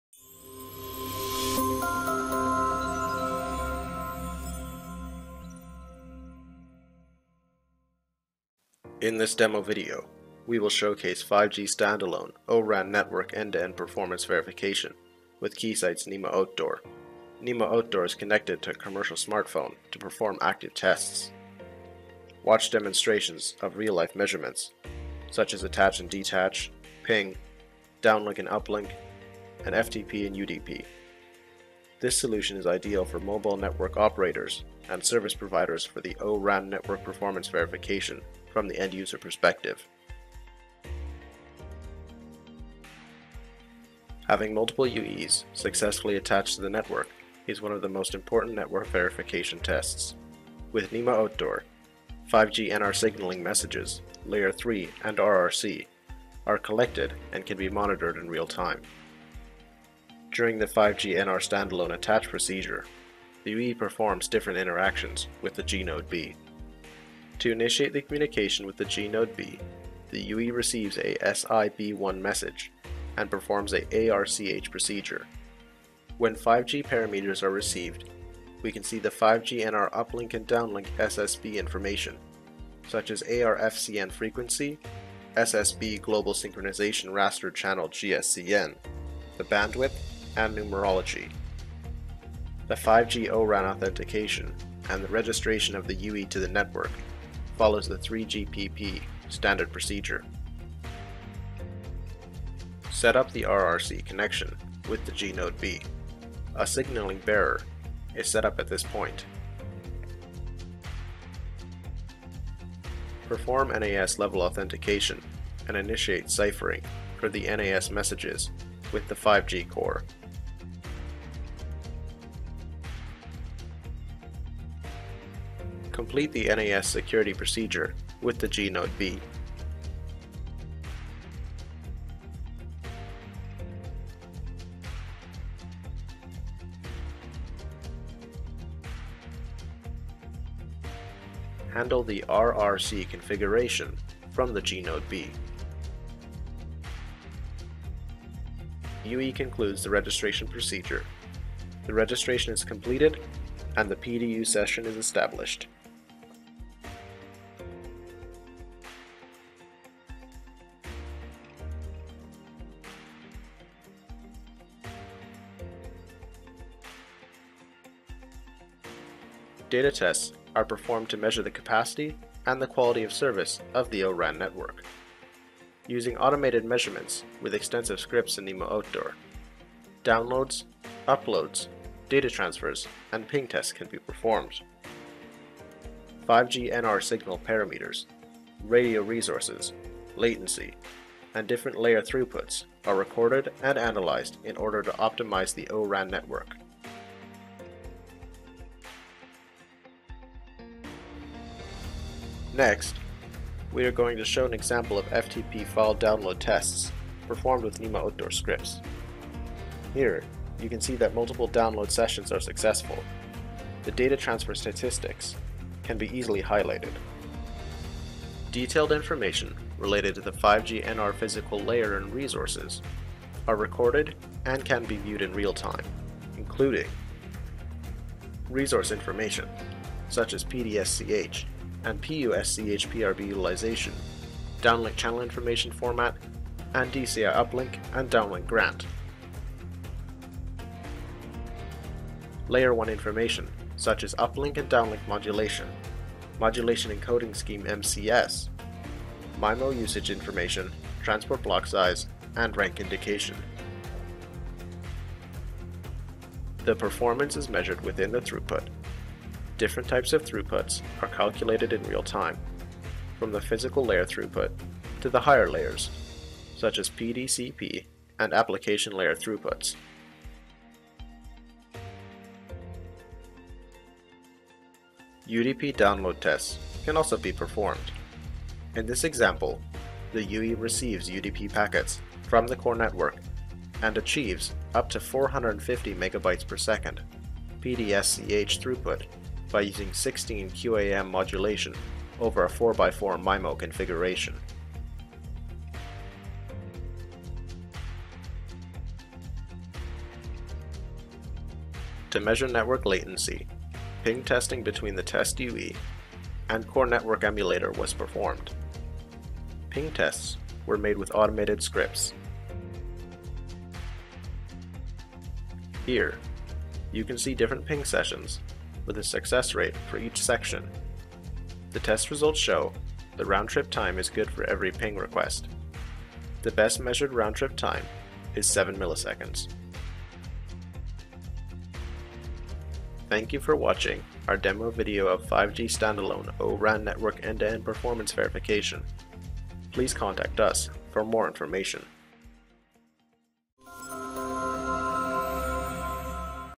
5G SA O-RAN Network E2E Performance Verification
The demo video showcases 5G SA O-RAN network end-to-end performance verification with Keysight’s Nemo Outdoor. Nemo Outdoor is connected to a commercial smartphone to perform active tests. Watch demonstrations of real-life measurements, such as Attach/Detach, Ping, and data transfers (FTP, UDP). This solution will be used by mobile network operators and service providers to verify O-RAN network performance from the end-user perspective.